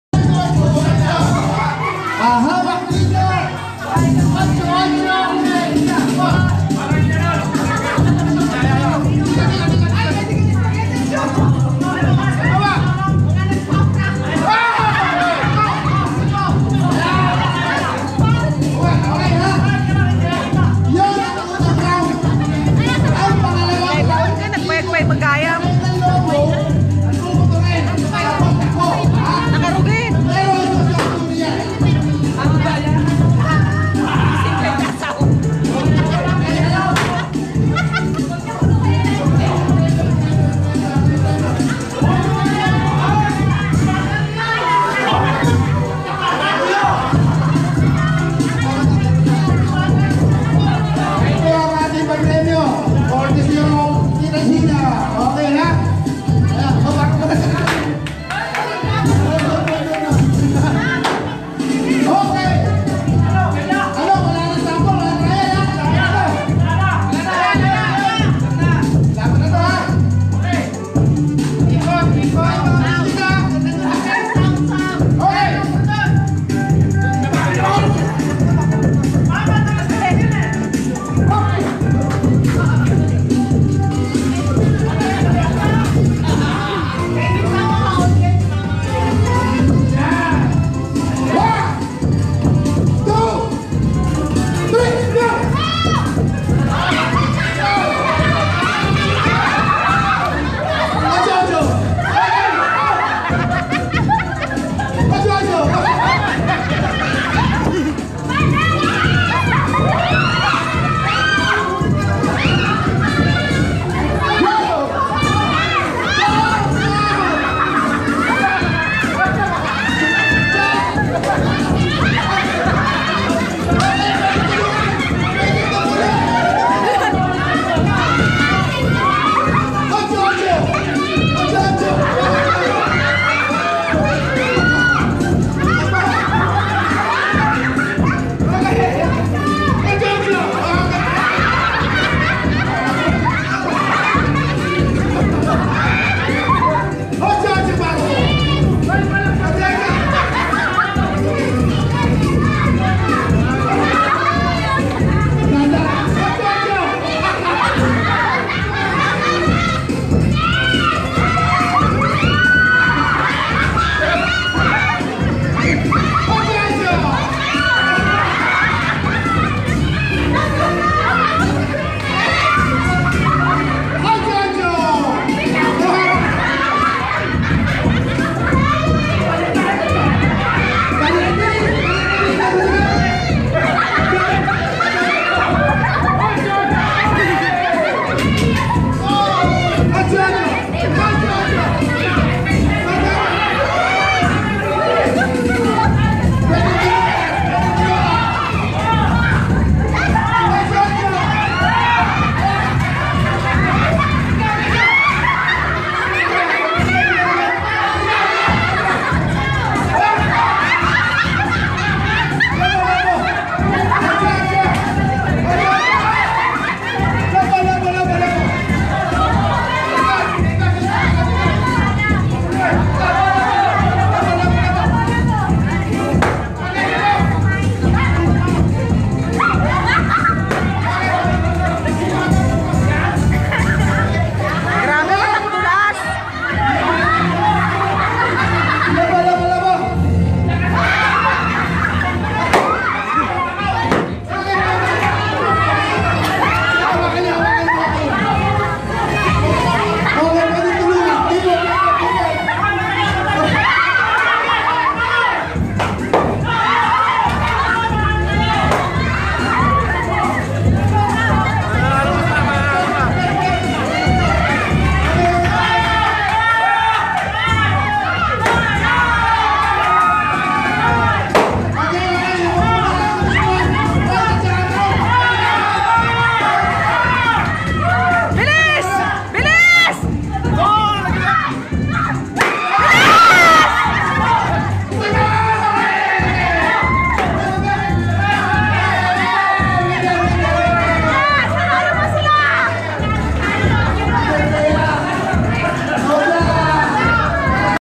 BASKETBALLERS Rome
a group of Basketballers in Rome having a great time.
di lang pang sports... pang ocho-ocho pa